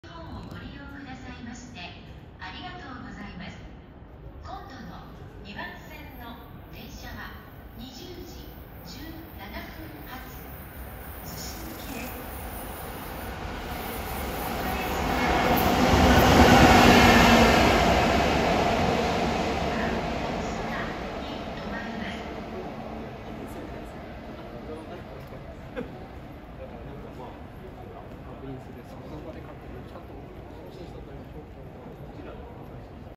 【E131系1000番台配給に伴う牽引機返却回送】国鉄EF64形1030号機が単機で東戸塚駅を高速通過するシーン（単9844レ）2023.12.14
ここしか間に合わなかった（）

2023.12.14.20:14